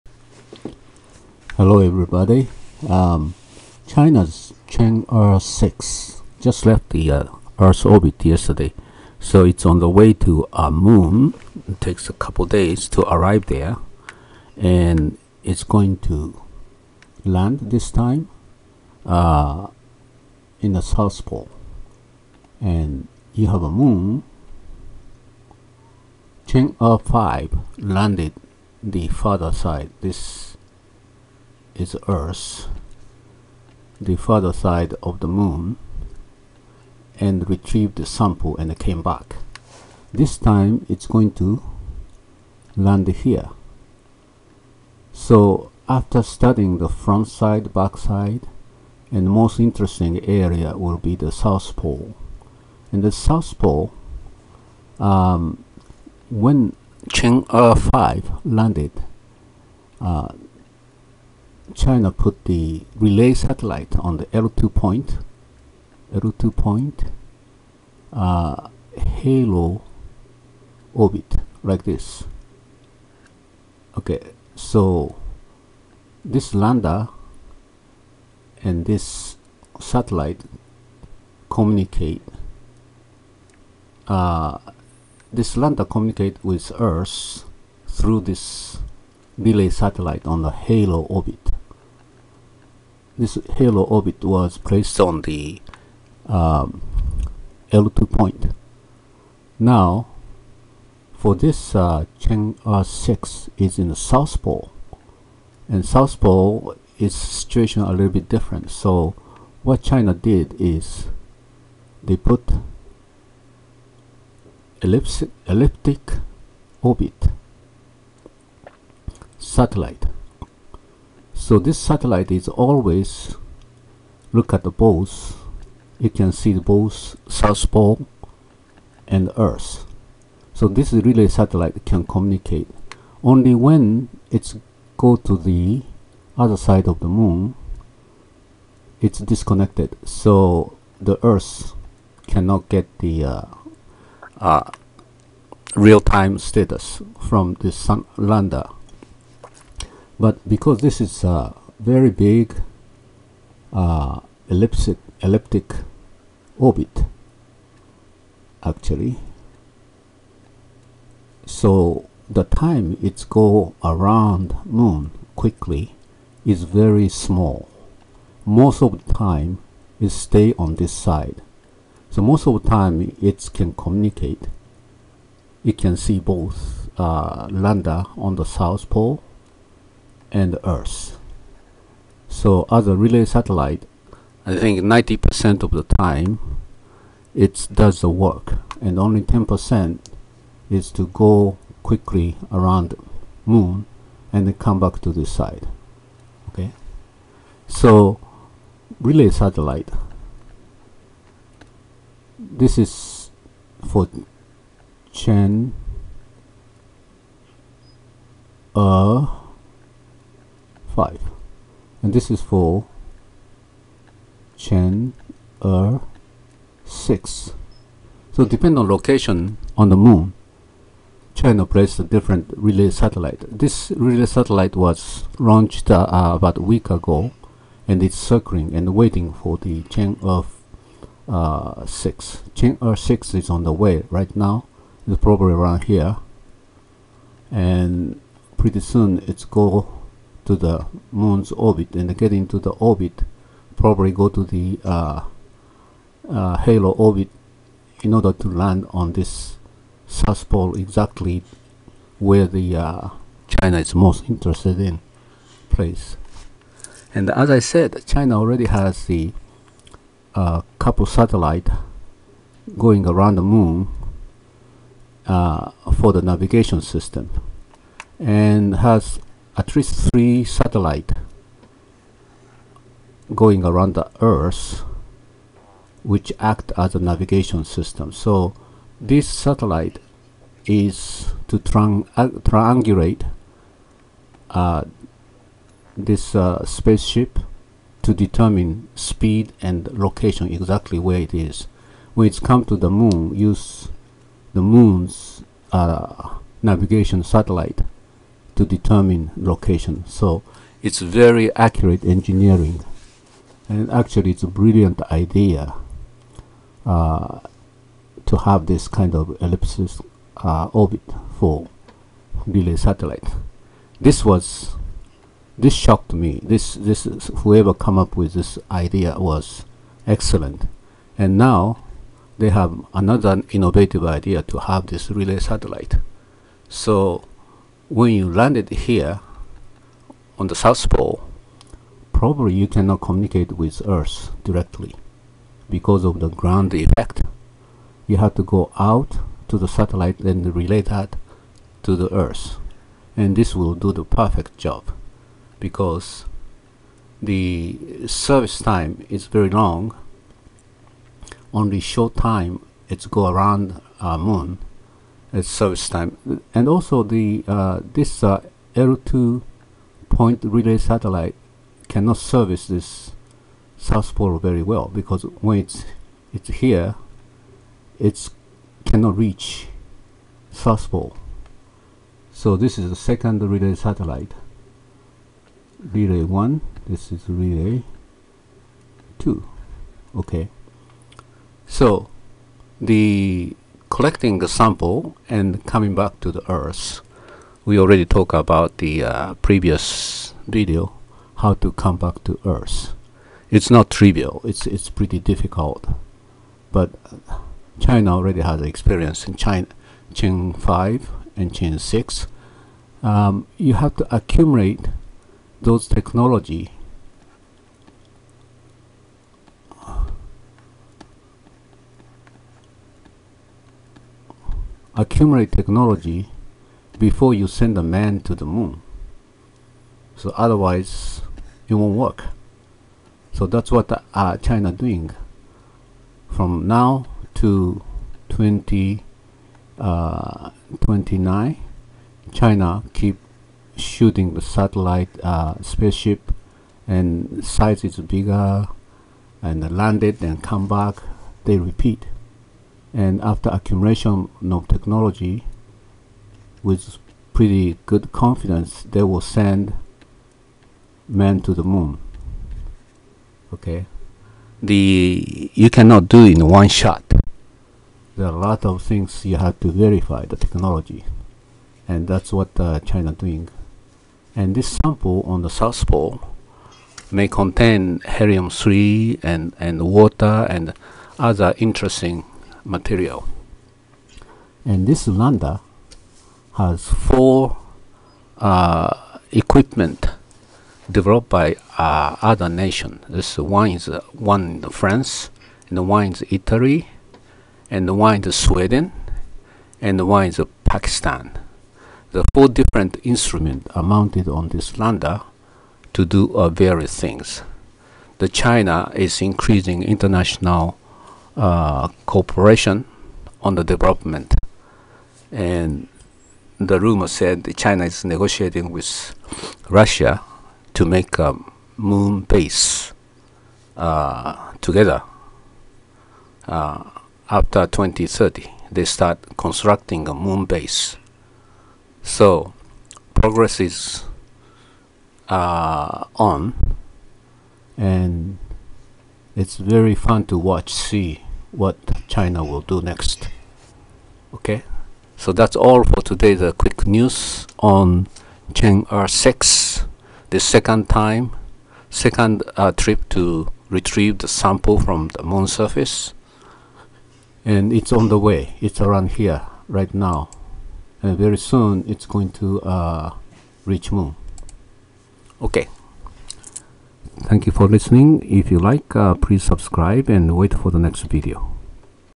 Dr. Lee's Math Series, Space Science - (8) Chang'e-6 is on the way to the moon
Quick report on Chang'e-6 schedule and technical details.